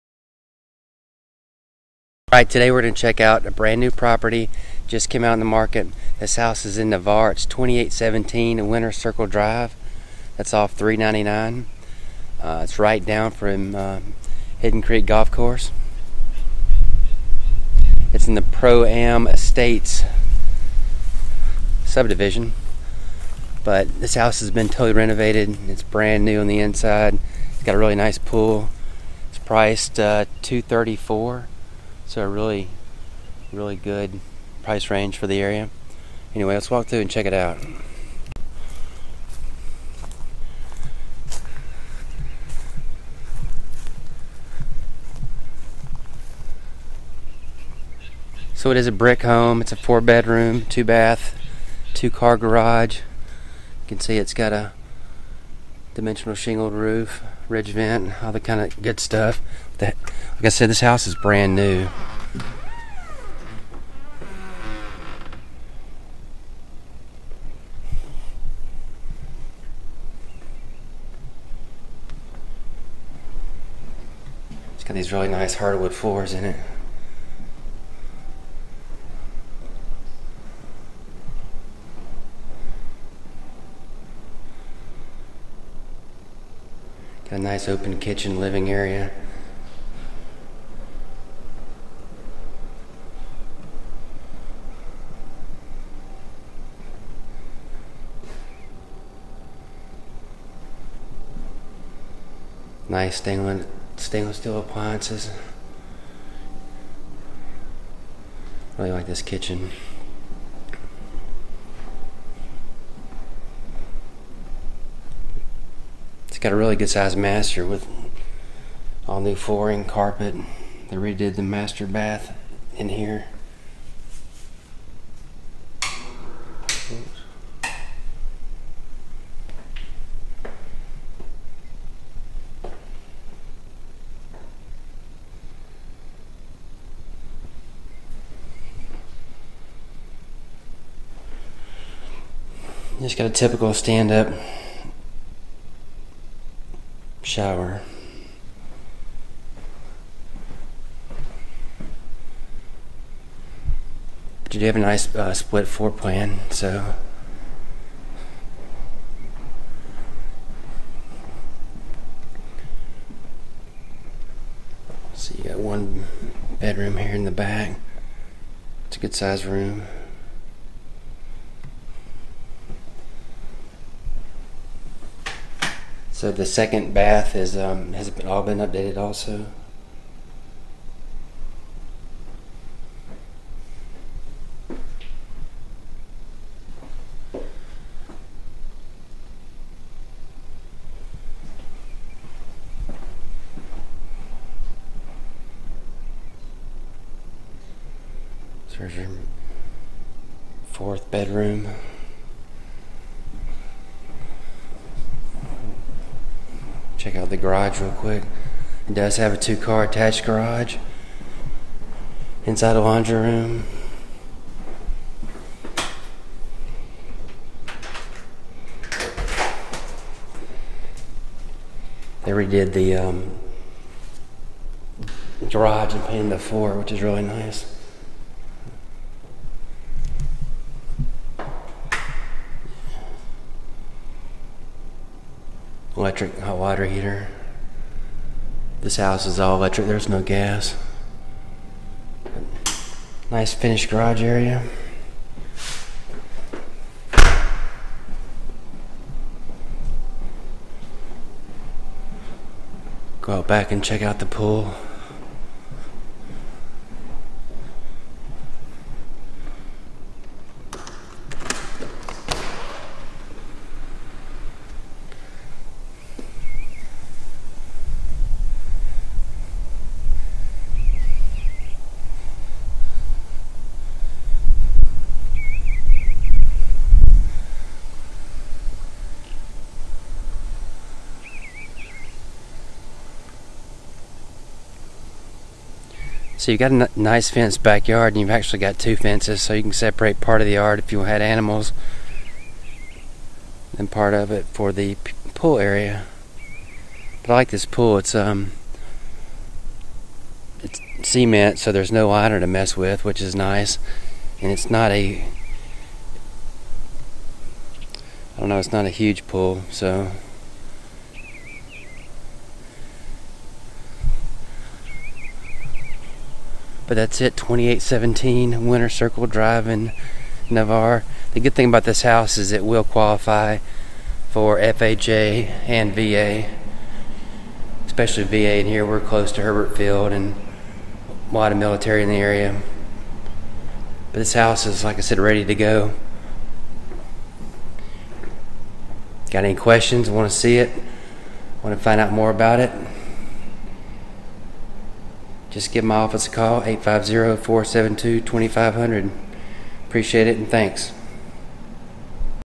For Sale: 2817 Winners Cir Rd Navarre, FL 32566
This home has been totally remodeled and has an amazing cement pool. Home features include new Granite counter tops in the kitchen and bathrooms, new carpeting, new appliances, the wooden floors were redone, freshly painted throughout and even has a very nice in-ground cement pool with a privacy fence.  *This one will also qualify for either FHA or VA financing.